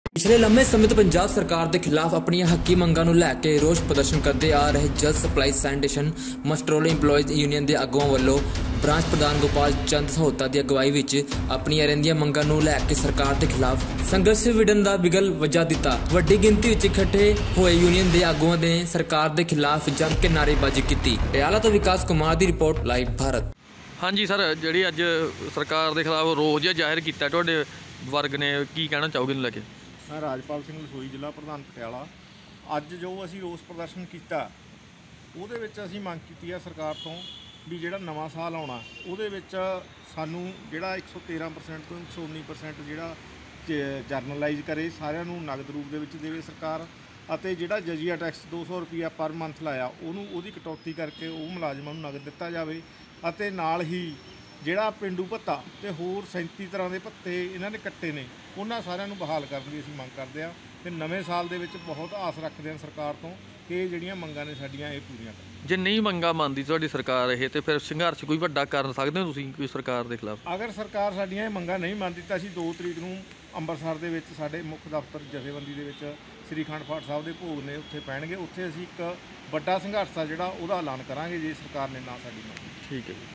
ਜਲ ਸਪਲਾਈ ਯੂਨੀਅਨ ਨੇ ਸਰਕਾਰ ਦੇ ਖ਼ਿਲਾਫ਼ ਆਪਣੀਆਂ ਹੱਕੀ ਮੰਗਾਂ ਨੂੰ ਲੈ ਕੇ ਰੋਸ ਜਾਹਿਰ ਕੀਤਾ
ਜਲ ਸਪਲਾਈ ਸੈਨੀਟੇਸ਼ਨ ਮਸਟਰੋਲ ਇੰਪਲਾਈਜ਼ ਯੂਨੀਅਨ ਦੇ ਪ੍ਰਧਾਨ ਗੋਪਾਲ ਚੰਦ ਸਹੋਤਾ ਦੀ ਅਗਵਾਈ ਵਿਚ ਸਰਕਾਰ ਦੇ ਖ਼ਿਲਾਫ਼ ਆਪਣੀਆਂ ਹੱਕੀ ਮੰਗਾਂ ਨੂੰ ਲੈ ਕੇ ਰੋਸ ਜਾਹਿਰ ਕੀਤਾ ਗਿਆ